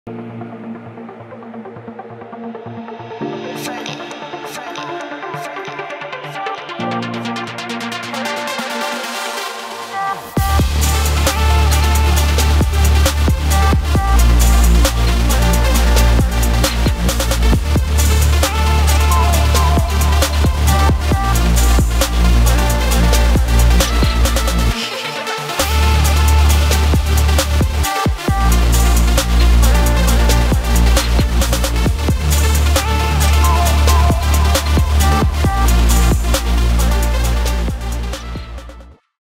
Mercedes G63 | Riviera RF108 | Manchester | G Wagon
Mercedes G63 G Wagon fitted with a set of our Flow Formed RF108 Wheels in 23x10.5 all round finished in Bronze Double Dark Tint. Our RF108 are also available in 19, 20 & 22", find out more at rivierawheels.co.uk